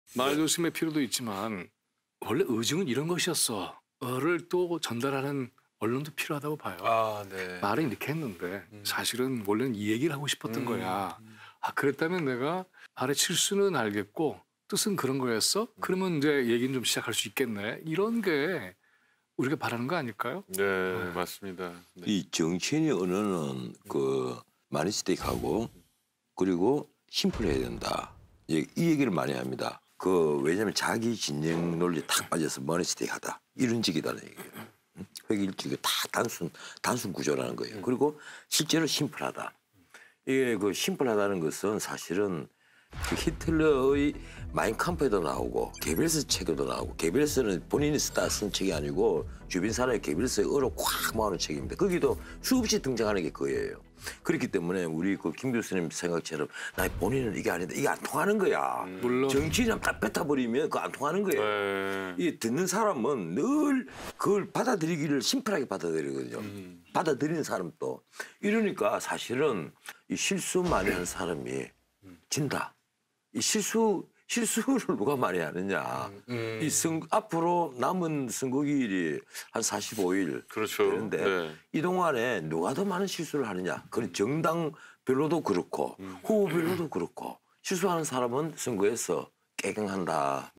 한국 정치, 제대로 된 언론이 필요하다? 김민웅 교수의 날카로운 지적!
정치인의 말조심도 필요하지만 이제는 진짜 ‘의중’을 전하는 언론이 필요한 때다? 김민웅 교수의 지적!